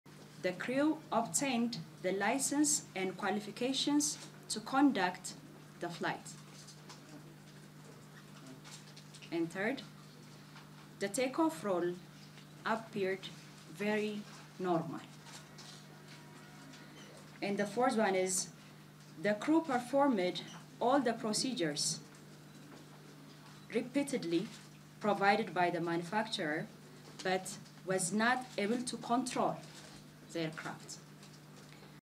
Ethiopian crew followed procedure, but unable to control jet
Ethiopian Transport Minister Dagmawit Moges says the crew of crashed Ethiopian jet 'repeatedly' performed Boeing's recommended procedures, during a press conference where officials released preliminary conclusions in the probe of last month's Boeing 737 crash that killed 157 people. SOUNDBITE from Ethiopian Transport Minister Dagmawit Moges